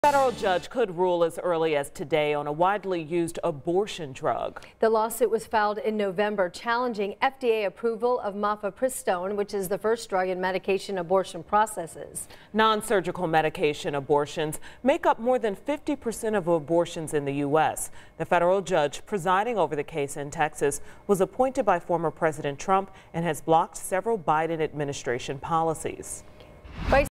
Abortion drug ruling expected soon
A federal judge could rule as early as today on a widely-used abortion drug.